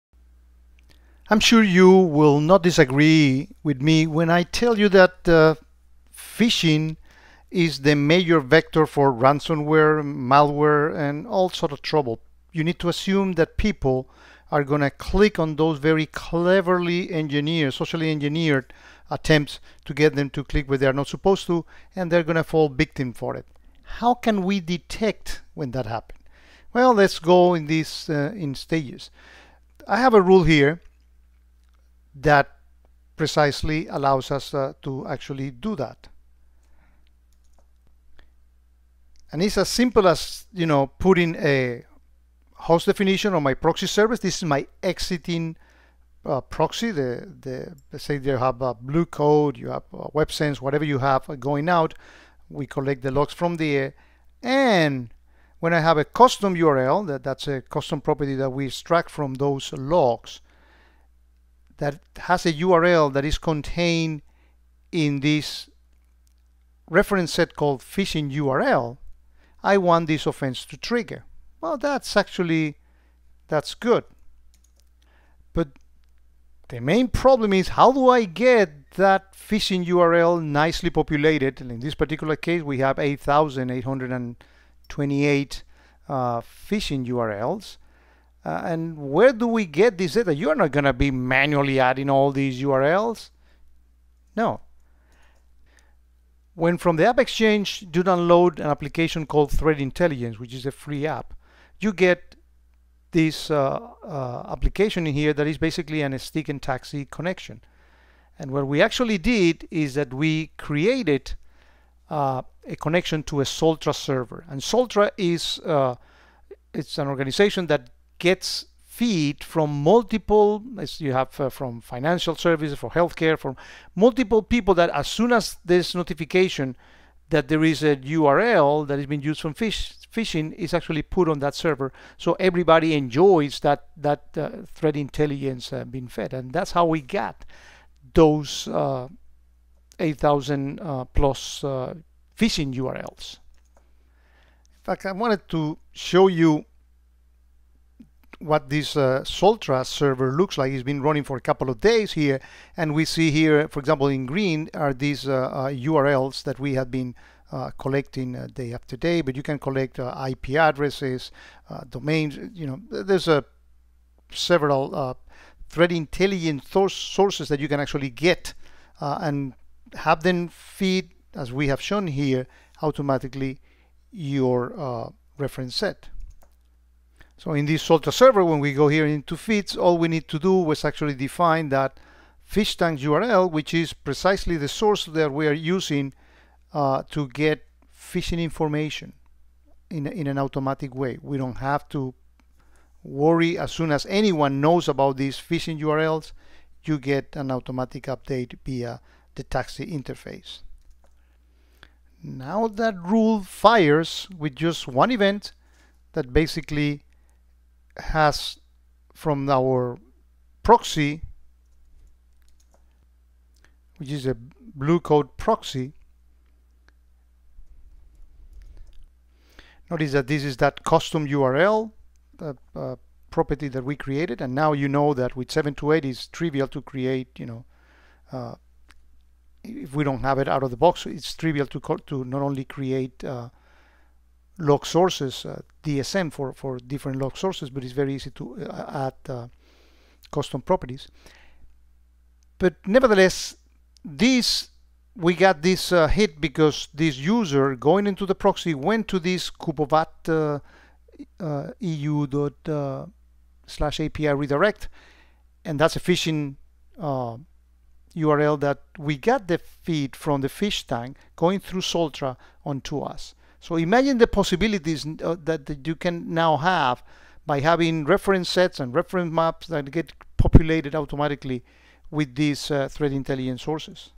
Populating Reference Sets from Soltra and other threat sources
How to populate a reference set for phishing URLs from a Soltra feed.